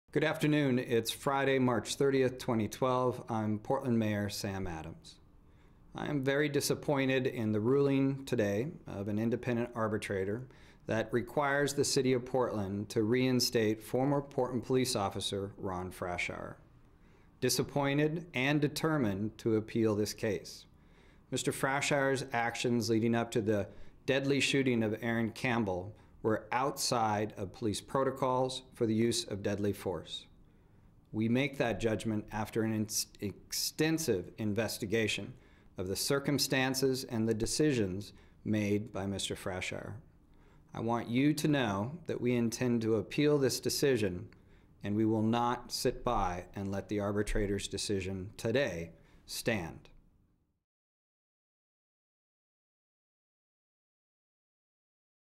Statement from Mayor Sam Adams and Police Chief Mike Reese Regarding Frashour Arbitrator Ruling
The City of Portland and the Portland Police Bureau have been informed by Arbitrator Jane Wilkinson that she has ruled in favor of the grievant, Ron Frashour, who was terminated from the Portland Police Bureau on November 16, 2010, following the January 29, 2010 shooting of Aaron Campbell.